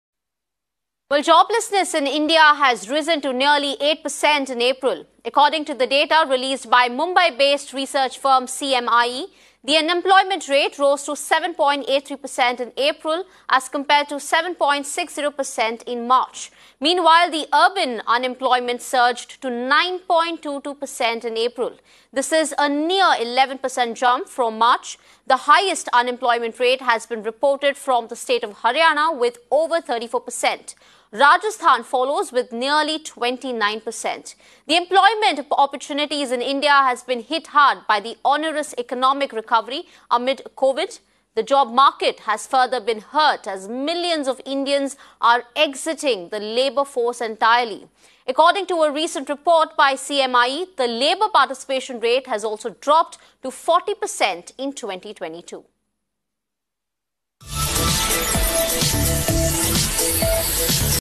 India Faces Massive Urban And Rural Unemployment After The Economic Dip Caused Due To Covid
After the Covid 19 pandemic, the economic condition of India took a steep dip. This closed employment opportunities for many people both from the urban and rural sectors. But the conditions don't seem to be getting better. In April, the rate of unemployment reached 8% according to the Mumbai-based research firm CMIE. In March, the numbers read 7.6. Meanwhile, the Urban unemployment rate surged to 9% in April. The highest unemployment rate has been reported in the State of Haryana at 34%. Rajasthan follows with 29%. Overall job opportunities in the country have been low and a major employment crisis can be seen.